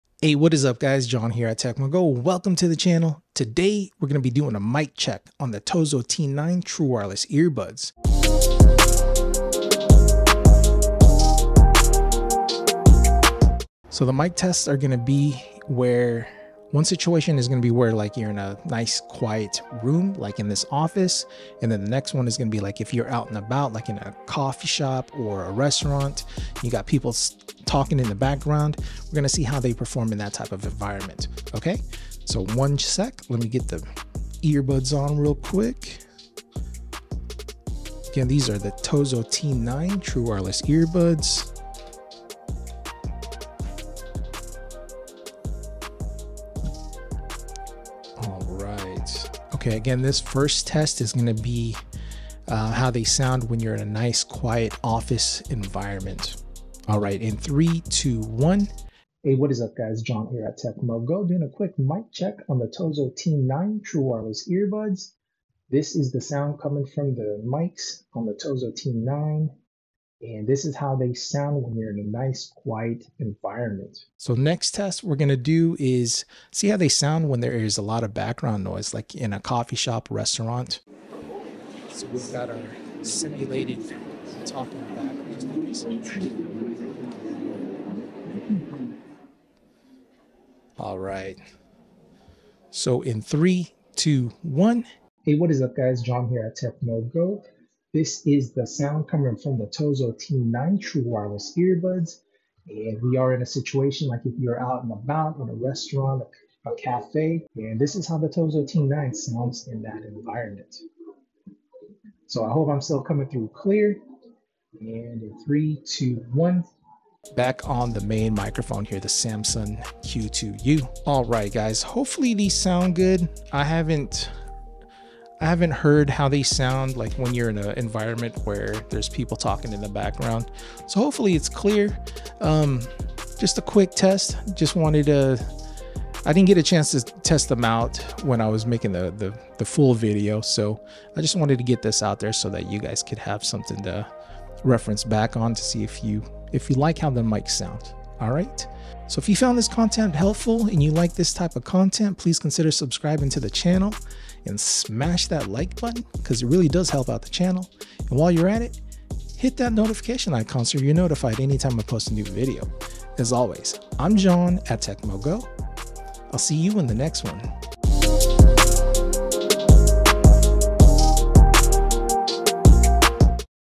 Mic Check on the TOZO T9 TWS earbuds
The Tozo T9 True Wireless Earbuds features Bluetooth 5.3, Environmental noise cancellation, fast charging and IPX7 water resistance. These pack a lot of heat without breaking the bank! Link below if you want to check them out!

My MacBook Air M1

Camera Gear:
Camera Canon M50:
Sony Zv-E10:
Samson Q2U Mic:
FiFine Mic:
Manfrotto compact tripod:
Manfrotto mini tripod: